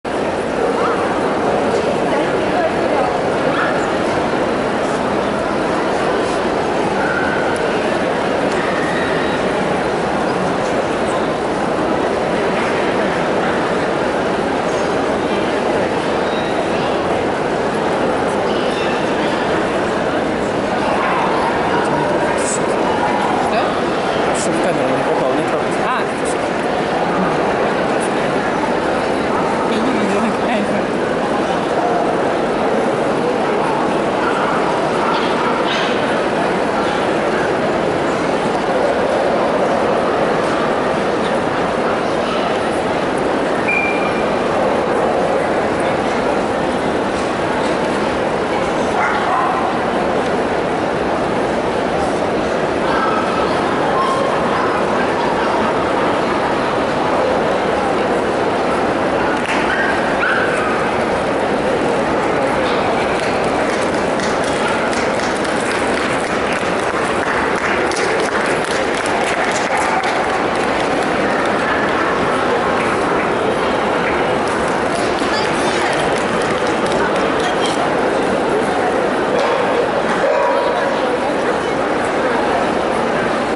"Eurasia-2" in Moscow 22 March 2015. The ring Bichon Frise.
Compare JBOB.
Expert - Jean Francois Vanaken (Belgium).